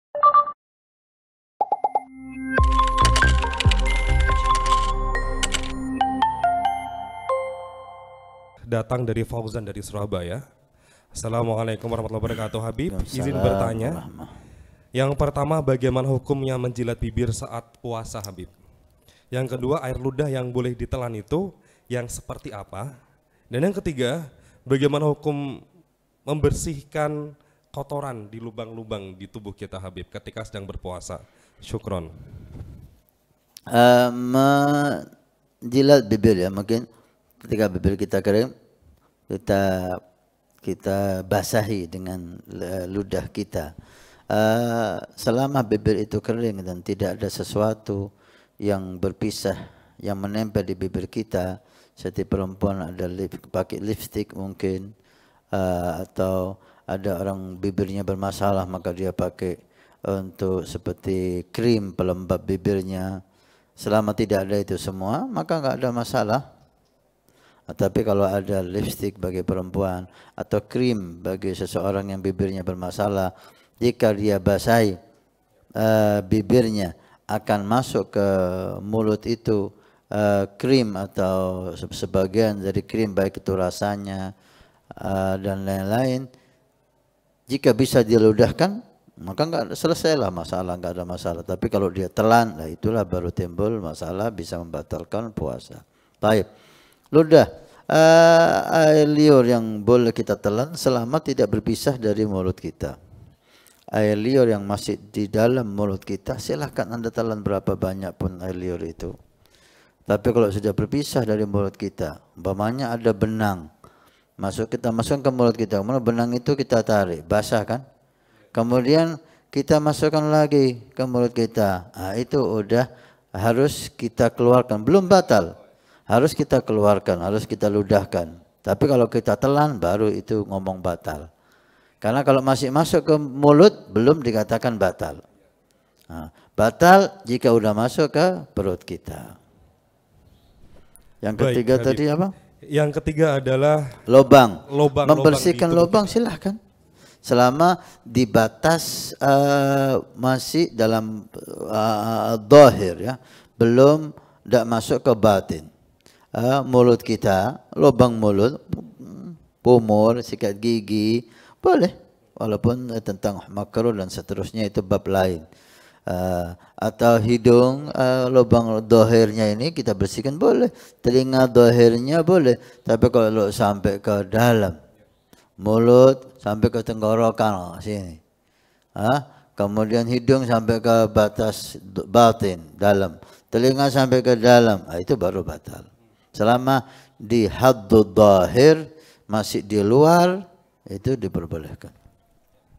Menjilat Bibir Saat Puasa - Habib Hasan Bin Ismail Al Muhdor
Pertanyaan:Habib Bagaimana Hukumnya Menjilat Bibir Saat Puasa?

Dalam video tanya jawab di studio Alwafa Tarim kraksaan, Habib Hasan menjelaskan tentang Menjilat Bibir Saat Puasa

Ada pertanyaan seputar agama dan kehidupan sehari - hari yang ingin diajukan ke Habib Hasan? silahkan DM di instagram @ahbaabulmusthofa_channel. Insyaallah pertanyaan kamu akan dijawab di episode selanjutnya.